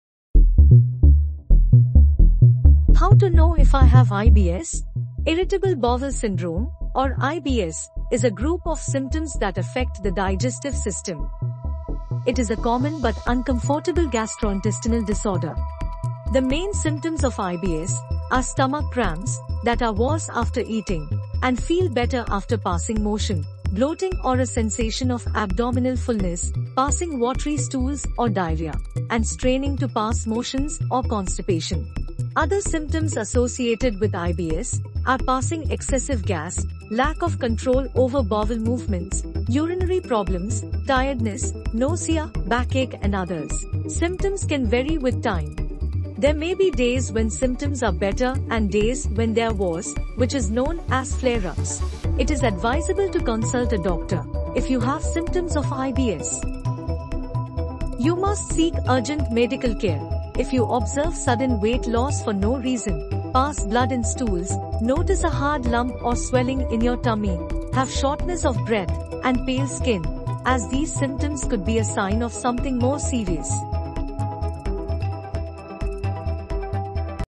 How To Know If I Have IBS?
Irritable Bowel Syndrome is a group of common gastrointestinal conditions such as diarrhea, constipation, stomach cramps, bloating, etc. Watch this video to learn more about the signs and symptoms of IBS and when to seek medical care for the same.